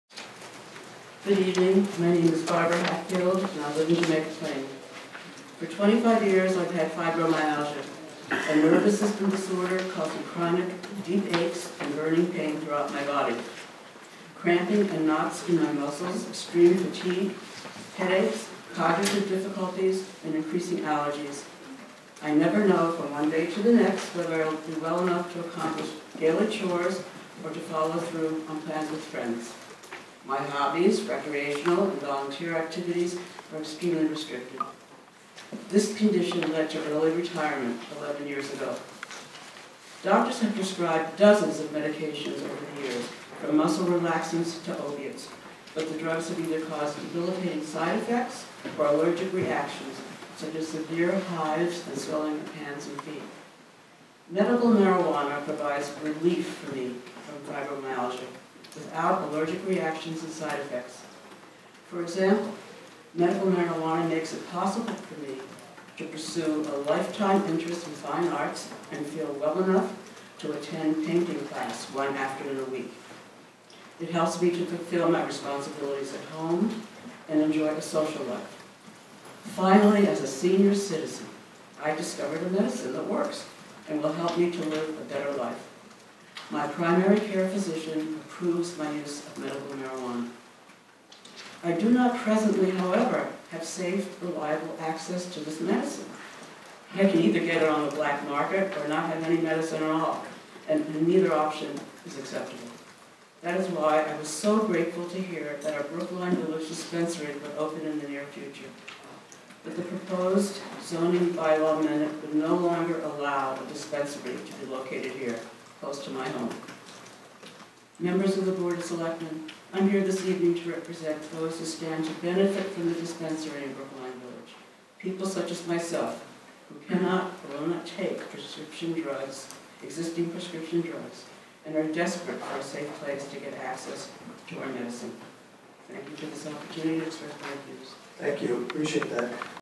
Barbara Hatfield - Patient Testimony - Brookline, MA
Barbara Hatfield - Patient with Fibromyalgia and Resident of Boston